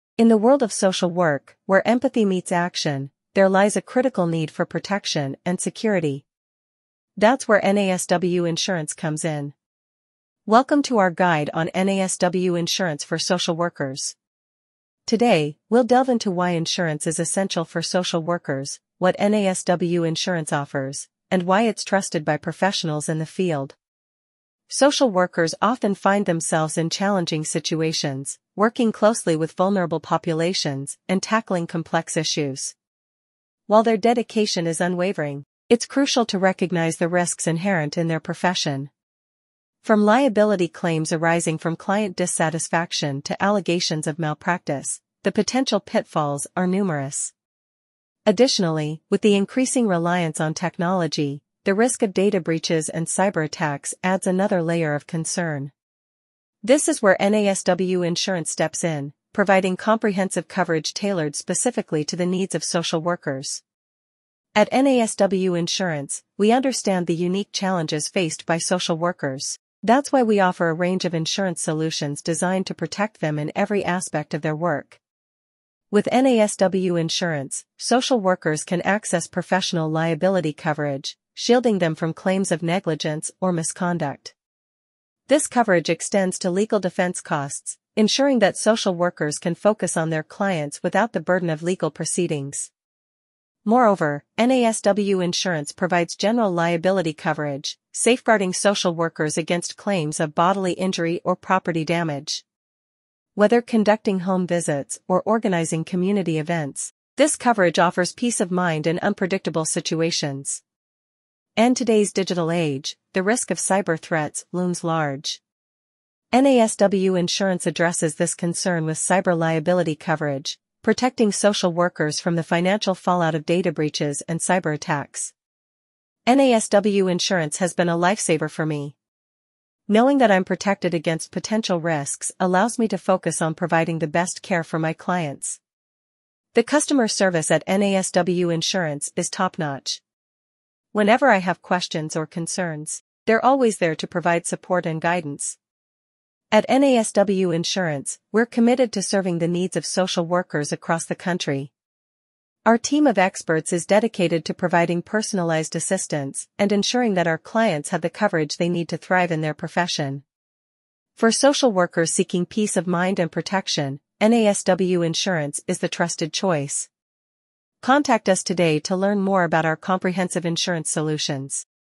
NASW Insurance for Social Workers
Embark on a journey of professional security with NASW Insurance for Social Workers in our latest video. Delve into the comprehensive coverage options tailored specifically for the unique needs of social work practitioners. Explore how NASW Insurance provides essential protection against potential risks and liabilities encountered in the field. From liability coverage to health and wellness benefits, witness how these insurance plans offer peace of mind and support for social workers as they fulfill their vital roles. Through expert insights and real-life examples, gain a deeper understanding of the specific benefits and advantages of NASW Insurance. Join us as we highlight the importance of NASW Insurance in safeguarding the integrity of social work practice, empowering practitioners to navigate their responsibilities with confidence and dedication. Discover how NASW Insurance is more than just a policy—it's a partner in ensuring the well-being and professionalism of social workers across the nation.